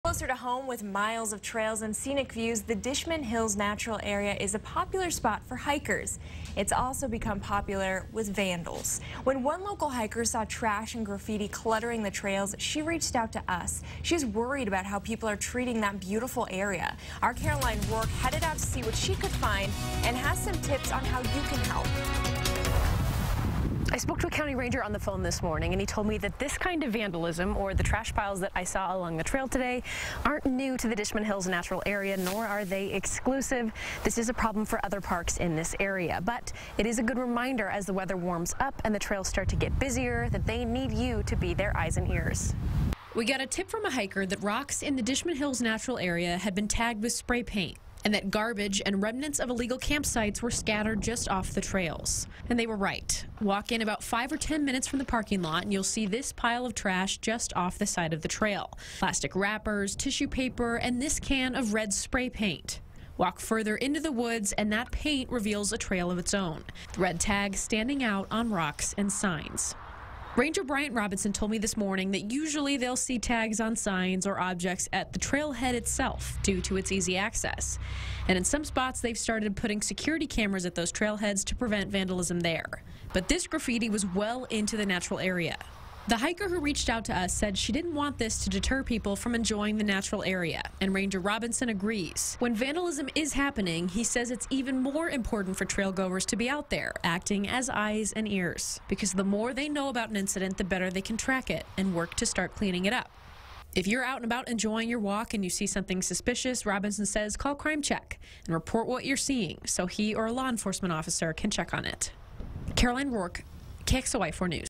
HIkers express concern about vandalism, garbage in Dishman Hills Natural Area
The Dishman Hills Natural Area is a popular spot for local hikers or dog-walkers looking for a place to get outside. It's also a popular spot for vandals